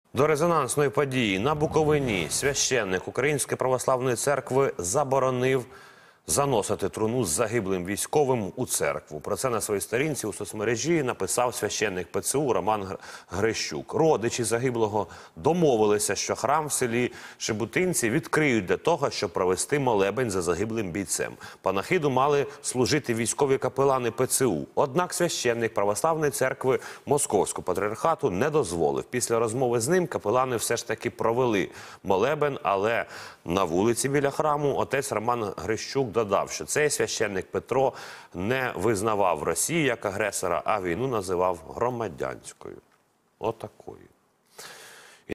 На Буковині священик УПЦ МП заборонив заносити труну з загиблим військовим у церкву
Священнослужитель зазначив, що бійця священики ПЦУ можуть відспівати на вулиці біля сільського клубу.